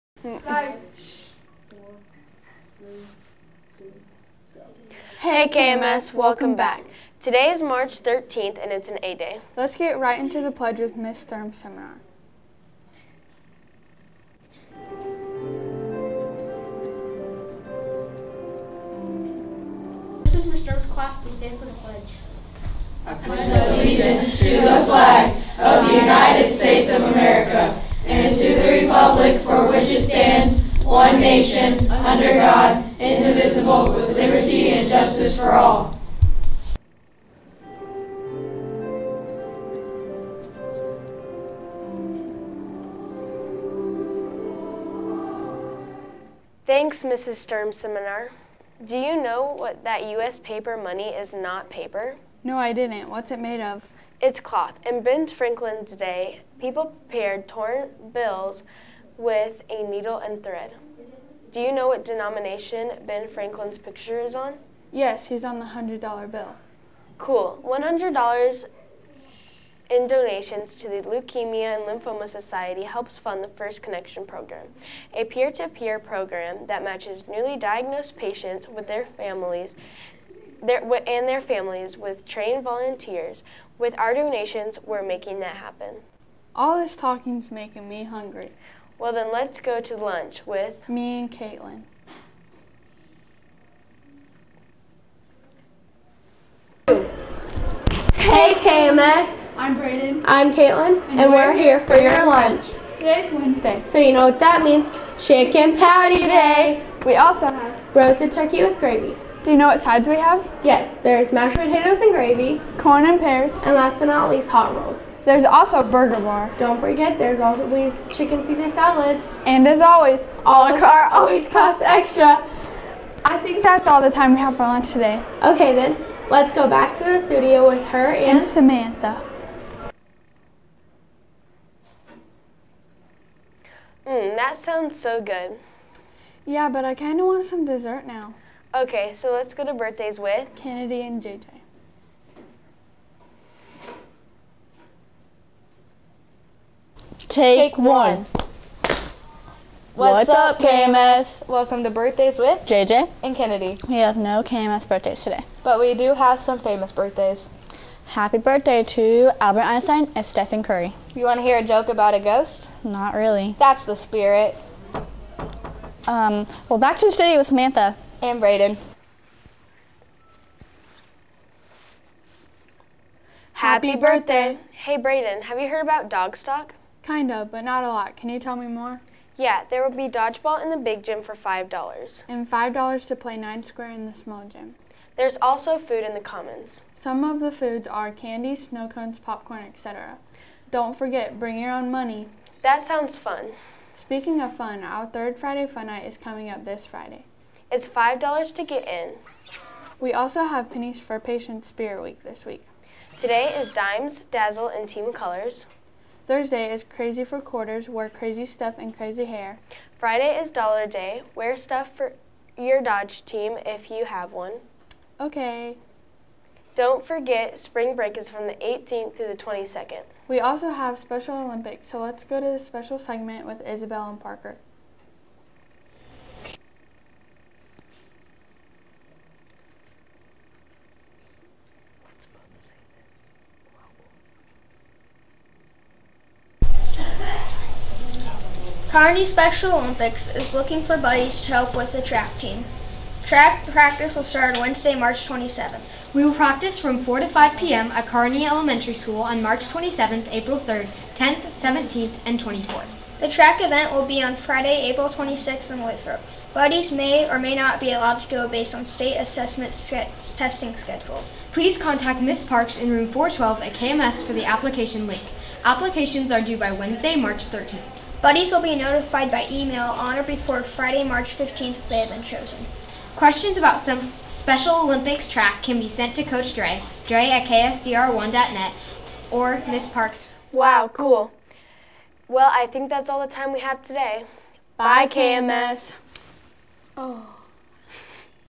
3 13 19
KMS Media 7 students write, film and publish the daily announcements - Bulldog Bulletin for 3-13-18  Season 12